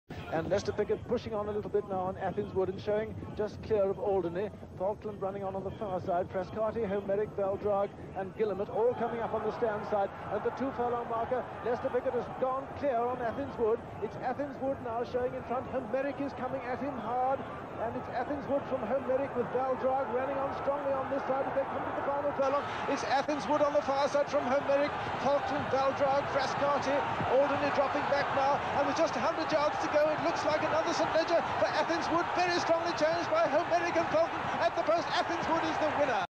Athens Wood - 1971 St. Leger
A power-packed ride from the front by Piggott.